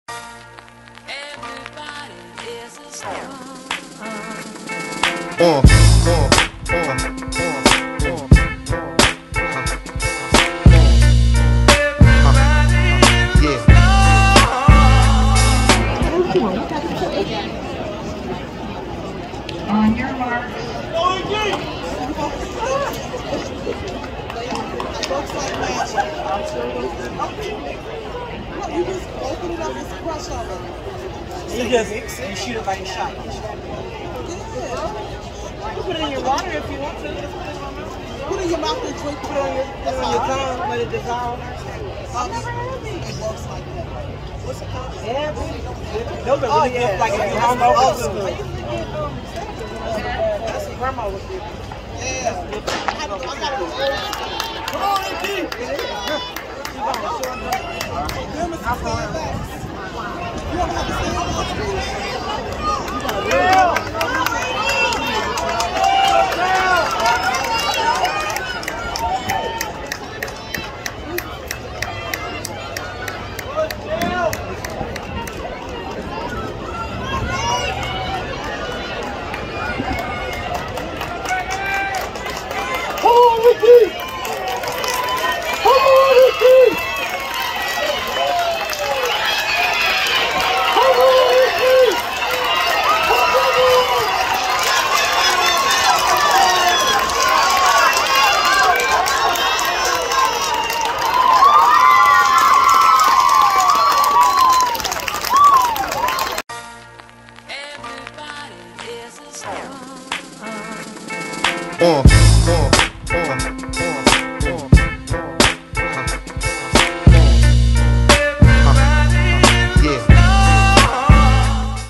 Boys 400 Meter Dash OHSAA Div 2 State Championship 2022
Watch as Brandy Burrey win a State Championship in the 400 Meter Dash by at least a 10 meter lead.